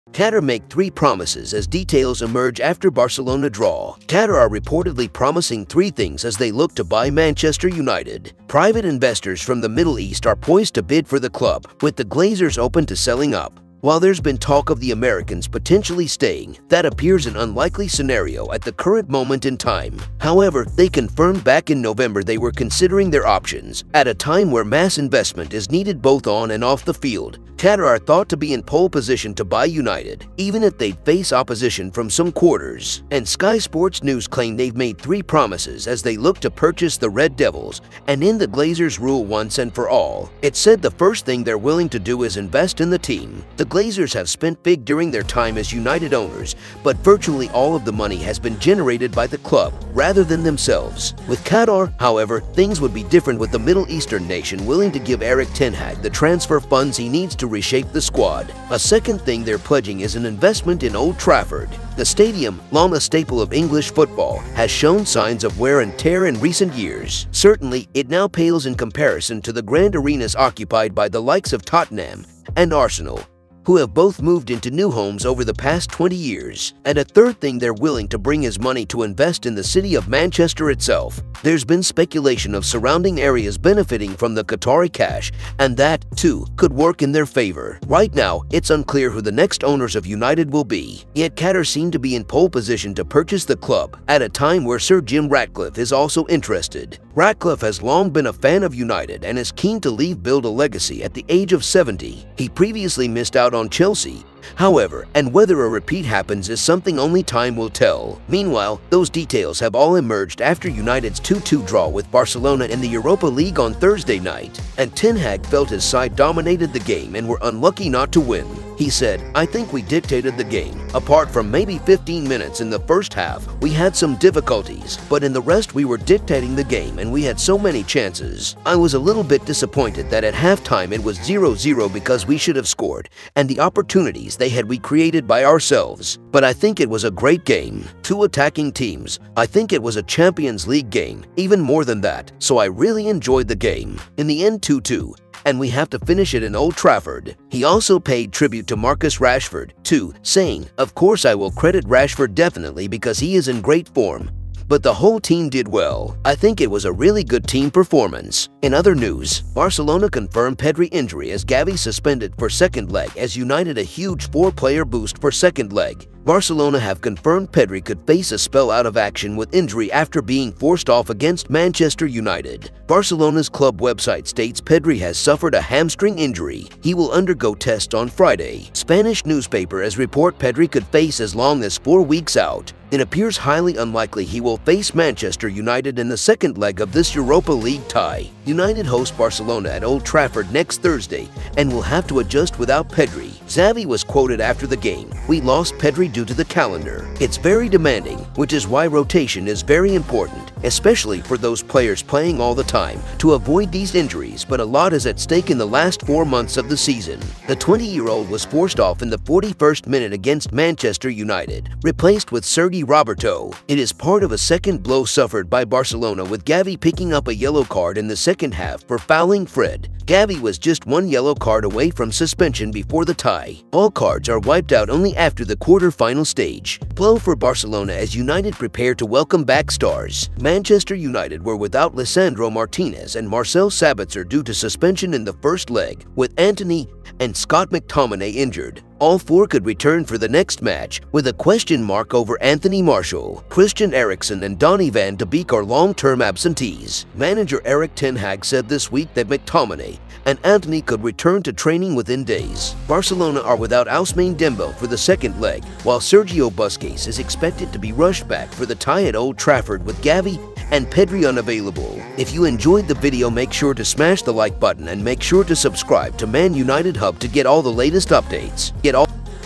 🛑QATAR'S 3 PROMISES TO UNITED!! As boss gets HUGE 4 player BOOST for baca return!!
Qatar make three promises as details emerge after Barcelona draw

Qatar are reportedly promising three things as they look to buy Manchester United. Private investors from the Middle East are poised to bid for the club, with the Glazers open to selling up. While there’s been talk of the Americans potentially staying, that appears an unlikely scenario at the current moment in time.